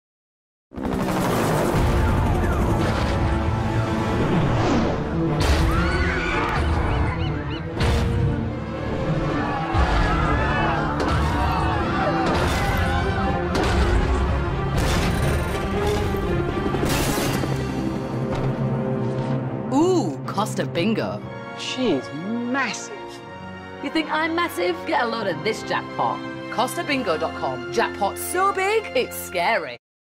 Giantess Mel B in Costa Bingo Commercial
In this video you see Giantess Mel B in a Costa Bingo Commercial.